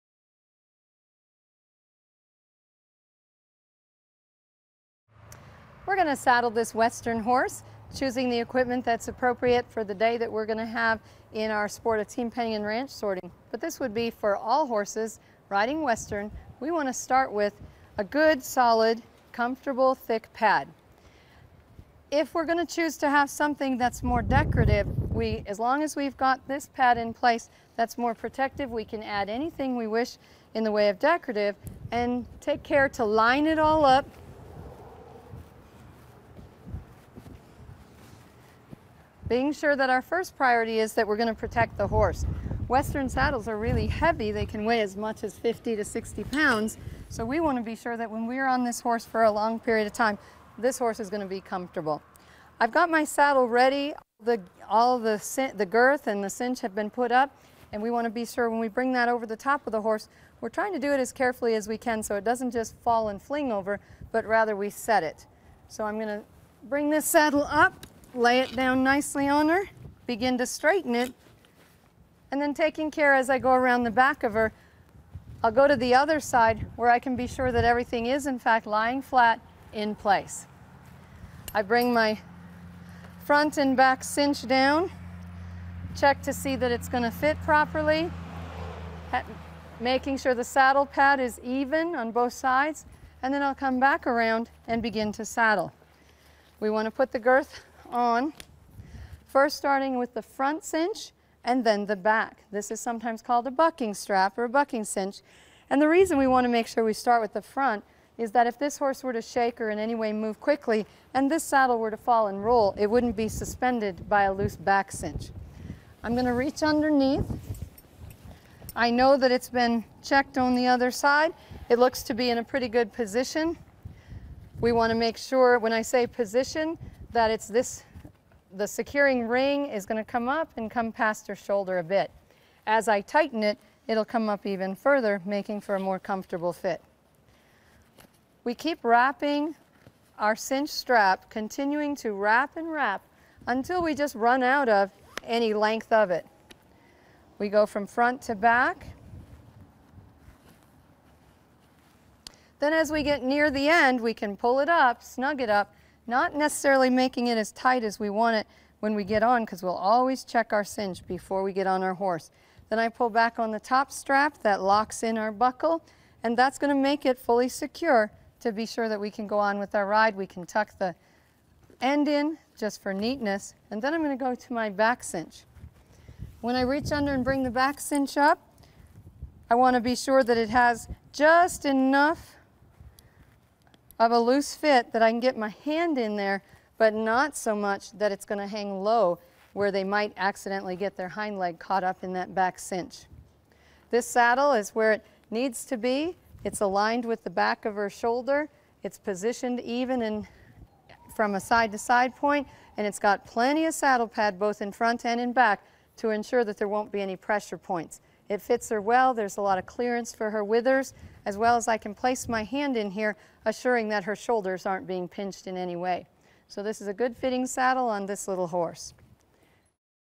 How to Saddle a Horse Western Style
Western-style riding requires a slightly different way of saddling a horse.  Learn how to saddle a horse for Western-style riding properly from a horseback riding instructor in this equestrian video.